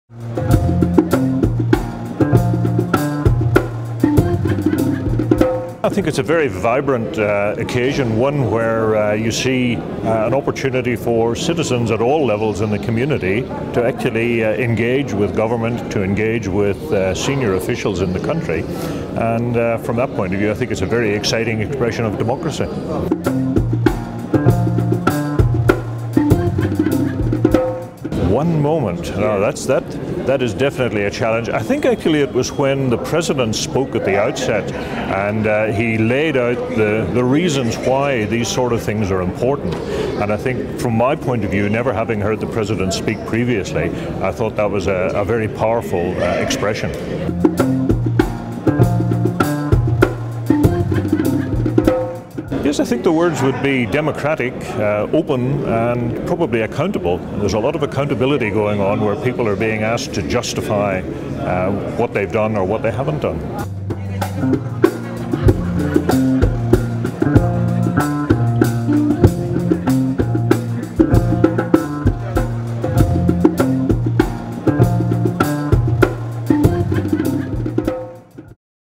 Umushyikirano 2013 - Interview with Professor James McWha from the University of Rwanda
The word Umushyikirano translates to a meeting where participants are able to exchange ideas, share experiences and question each other. Today Umushyikirano is known as the National Dialogue Council.

Umushyikirano aims to be a leading example of participatory and inclusive governance. It is hoped that by directly engaging with their leaders, Rwandans feel part of the decision making that affects their lives. Umushyikirano also serves as a forum for Rwandans to hold their leaders and government to account.

Umushyikirano is an annual event chaired by the President of Rwanda that gives Rwandans from all walks of life the opportunity to ask questions directly to their leaders. The event is attended by members of the Cabinet and Parliament, representatives of the Rwandan community abroad, local government, media, the diplomatic community and others.

In this interview, Professor James McWha from the University of Rwanda answers the following:

What does Umushyikirano mean to you?
What was the best moment?
Three words to define Umushyikirano?